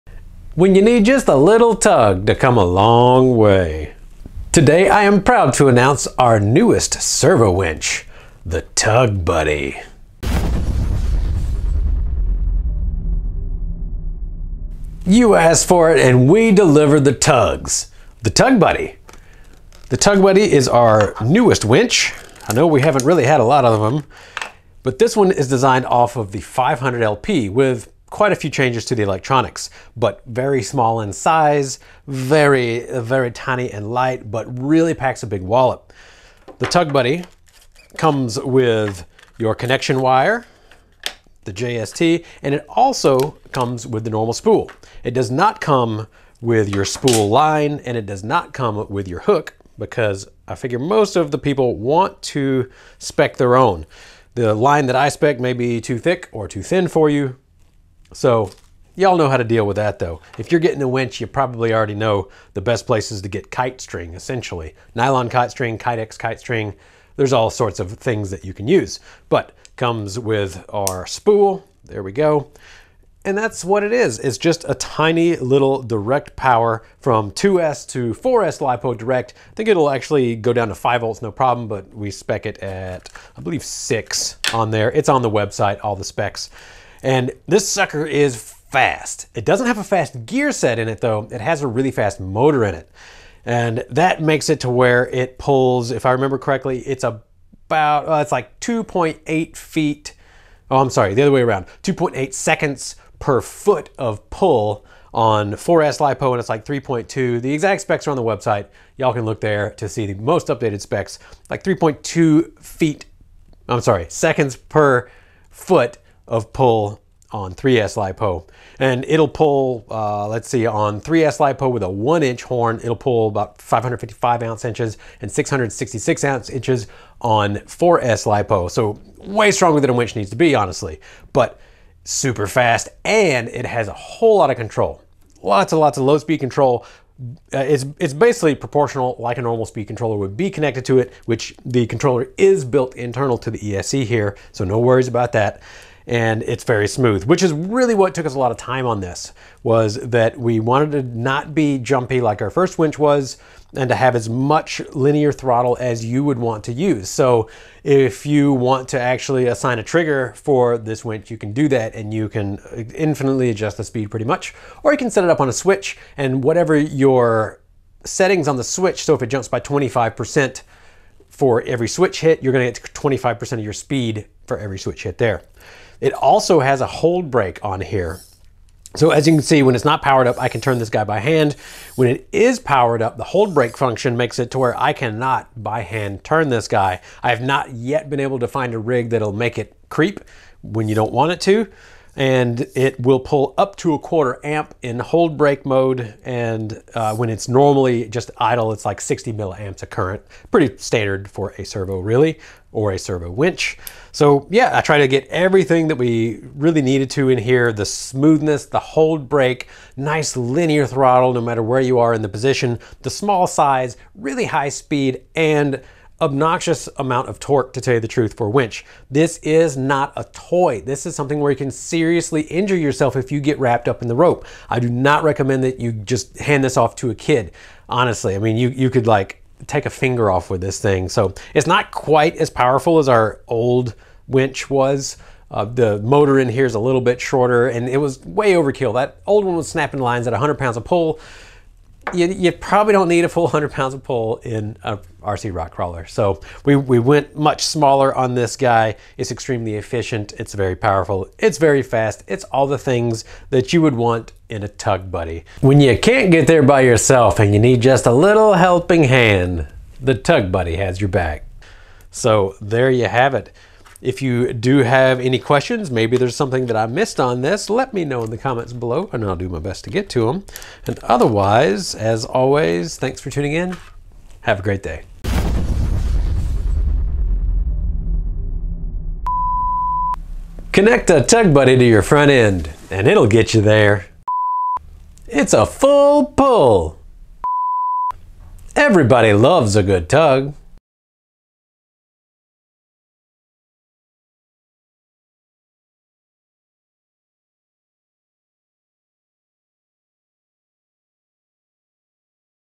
NEW SERVO WINCH from Holmes Hobbies (game changer) TUG BUDDY
Tug Buddy Low Profile 16v Brushless Servo Winch

This winch is not a toy! It has the power and speed to injure, not for children!!

Whether you just want to play on the trail or compete against the clock when weight and speed matter, the Tug Buddy has you covered! Its built into a tiny low profile case to save grams and size when it matters. Ultra efficient and power dense brushless motor, but with a very controllable and smooth tune to give you finesse when needed. The added hold brake feature will ensure you don't slip, no matter the angle or weight of vehicle!

Spool and JST power connector INCLUDED

FEATURES:

Includes Winch Spool 21mm diameter x 12.5mm thin
Includes JST power tap
Hold brake function
Water resistant Alloy Case with O-Ring Seals
Waterproofed internals
All steel machined 5 gear layout with industry leading strength
Ultra fast Brushless motor
30cm radio signal lead
2s to 4s LiPo battery compatible, 6 volt to 16.8 volt range
1 Year Warranty
SPECIFICATIONS:

Weight:69g
Dimensions: 40x20x30mm
Spline: 25 tooth (Futaba type) M3 threaded
11.1v spooling speed @ 3.3 seconds per foot (10 sec/ meter)
11.1v torque 555 oz/inch (40 kg/cm)
14v spooling speed @ 2.8 seconds per foot (8.5 sec/ meter)
 14v torque 666 oz/inch (48 kg/cm)